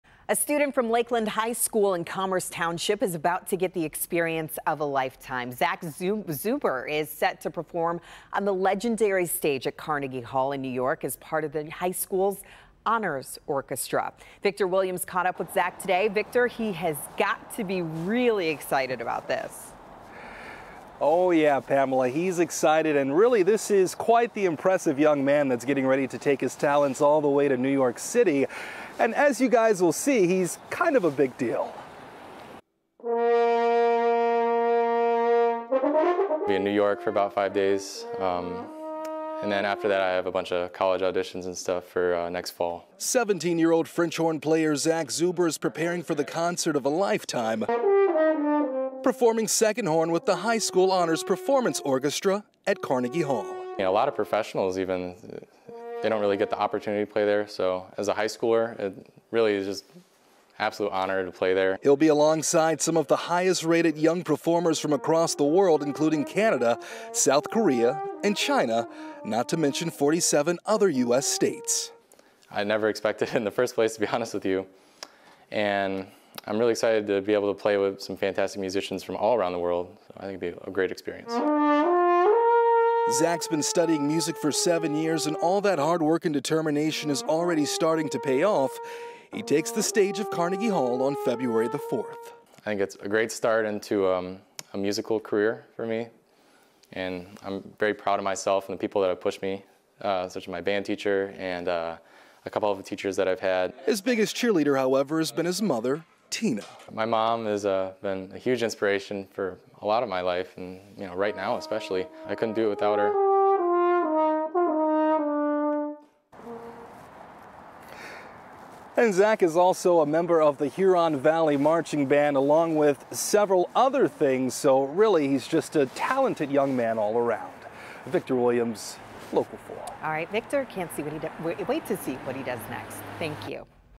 Commerce Township student set to perform at Carnegie Hall
A student from Lakeland High School in Commerce Township is about to get the experience of a lifetime. Zach Zuber is set to perform on the legendary stage at Carnegie Hall in New York City as part of the high school honors orchestra.